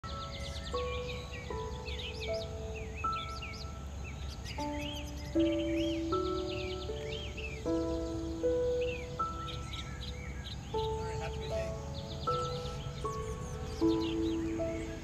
Mom doe and her cute yearlings ate all the mushrooms 🍄 from my backyard
Doe deer 🦌 and yearling eating 🥣 mushrooms 🍄 from my yard and swallowed them